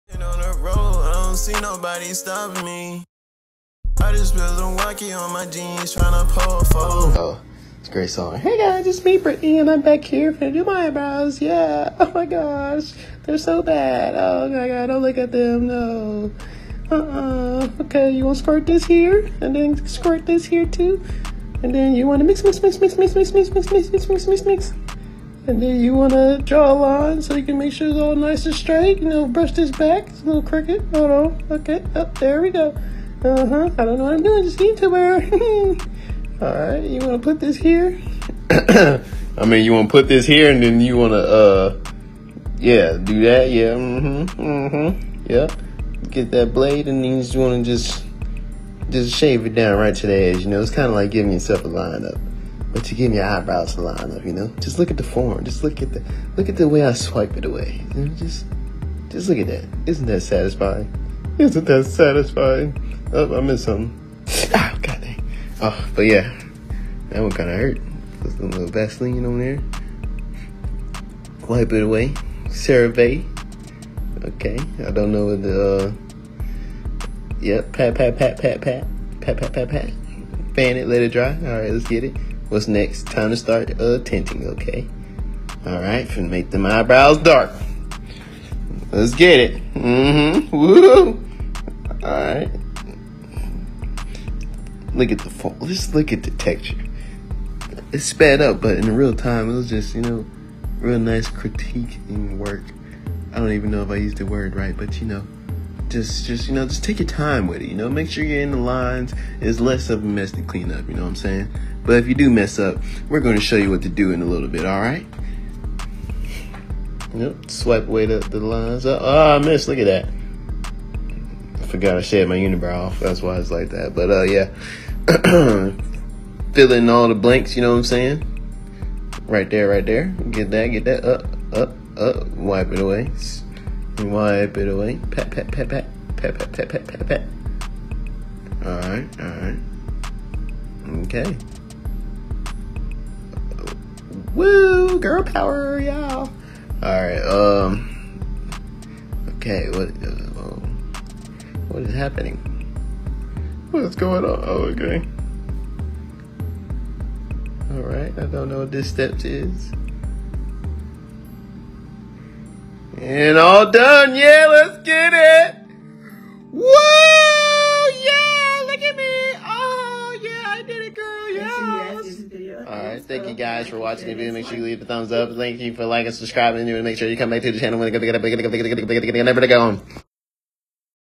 How to tint your Eyebrows for the Low 🤑| Brother does my voiceover!!! 🥴🤣
Product used:

Thank you for clicking on my video …